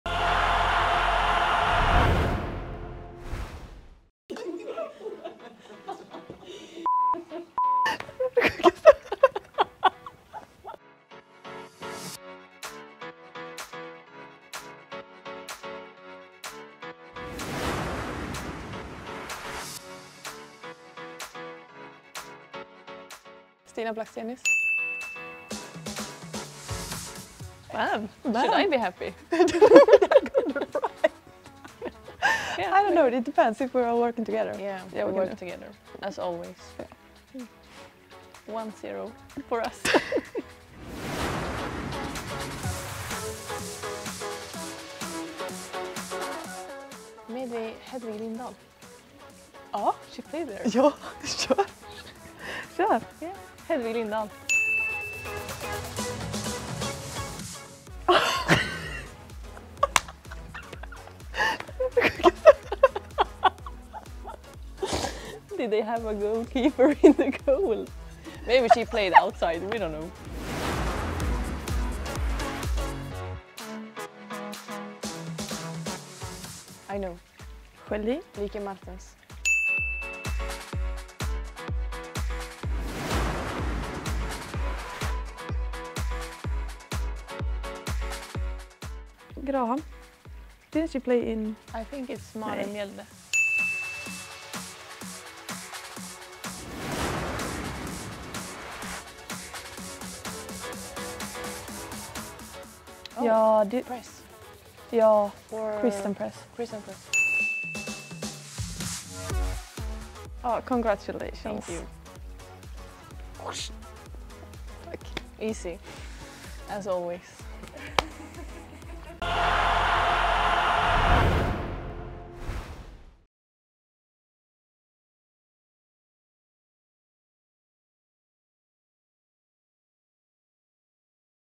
'Should I be happy?' - Anna Anvegård & Filippa Curmark Play Guess the Player
DAZN is proud to present a New Deal for women's football!